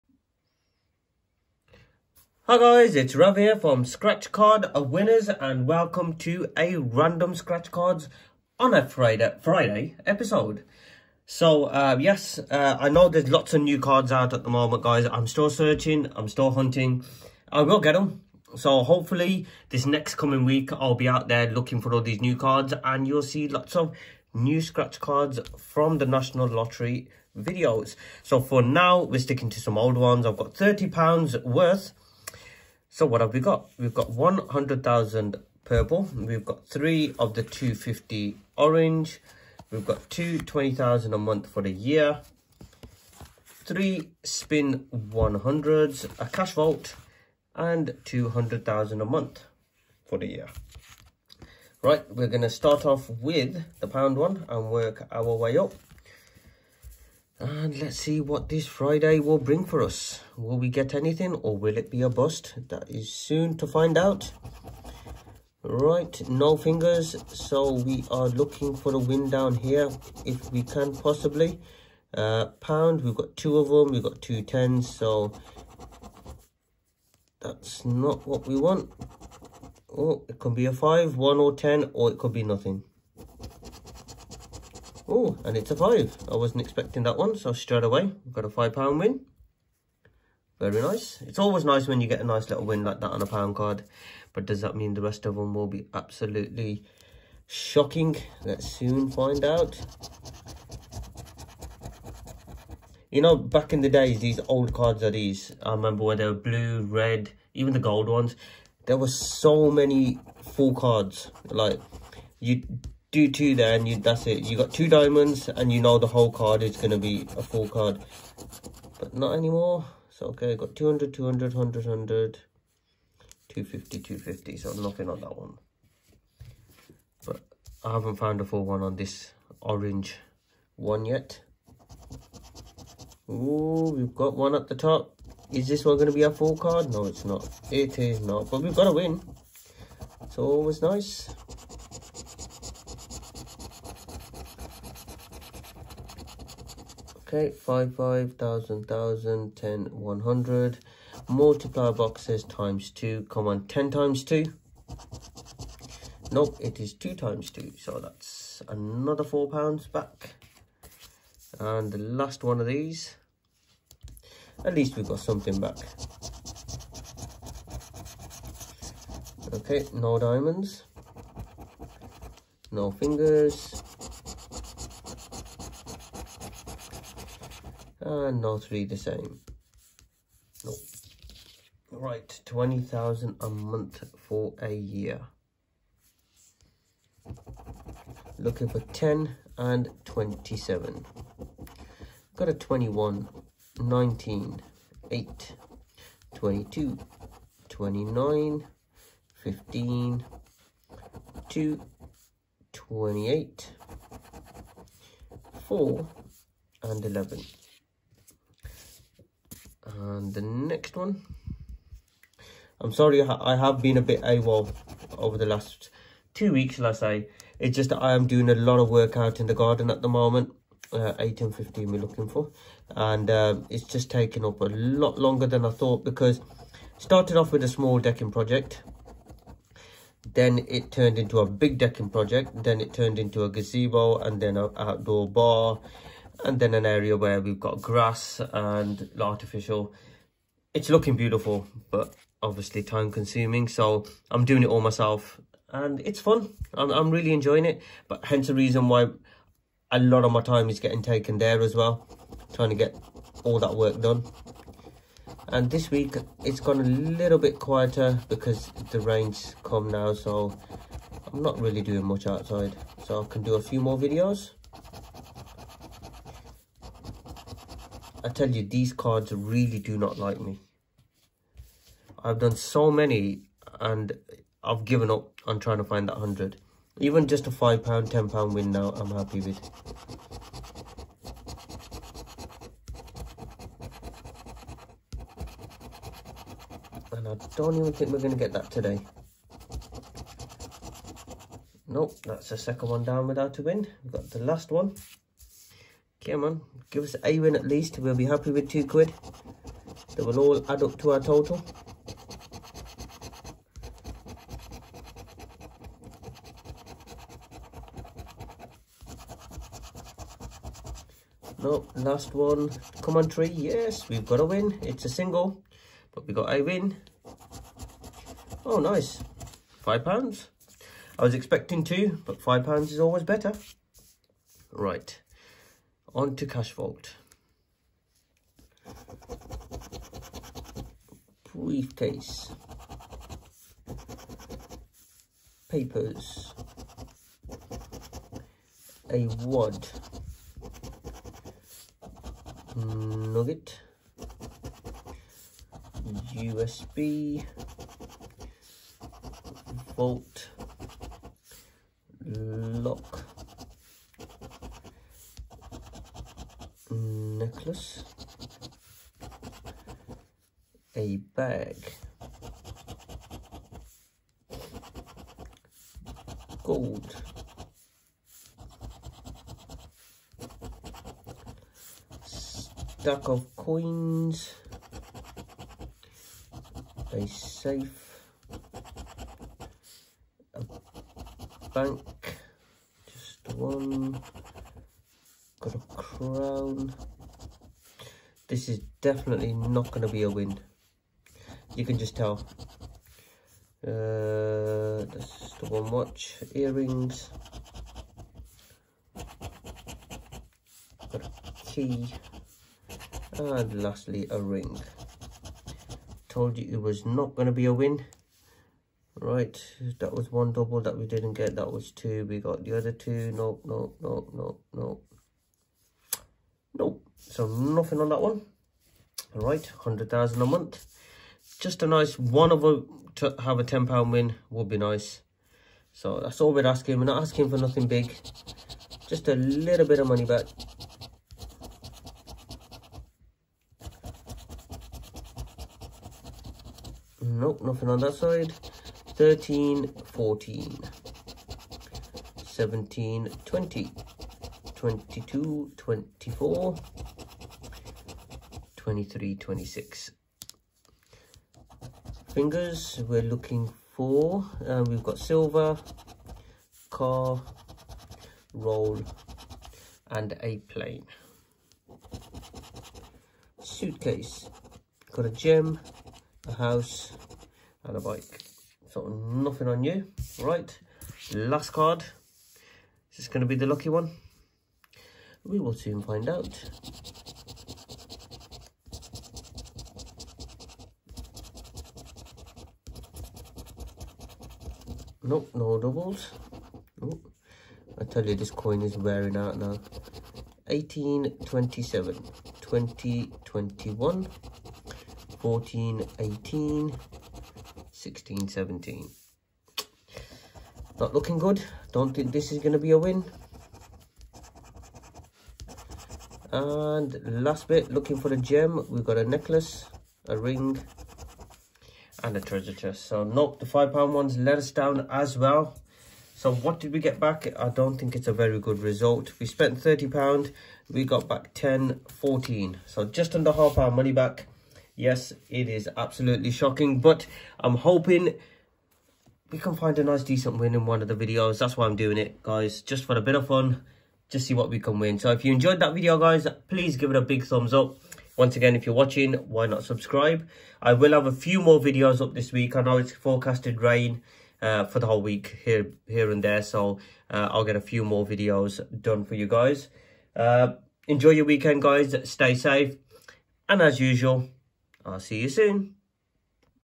Random Scratchcards On A Friday
Hi guys welcome to another Random Scratchcards on a Friday video we have a mix of £30 worth of Scratchcards lets see what will we win

Scratchcard winners Facebook page